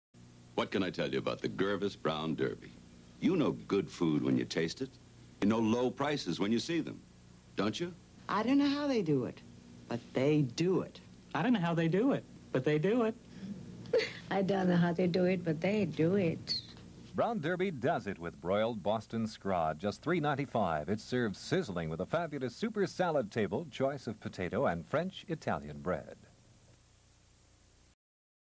Girves Brown Derby Restaurant 1978 TV commercial
Be on the look out for a Gabor sister in this 1978 television spot for Girves Brown Derby restaurants.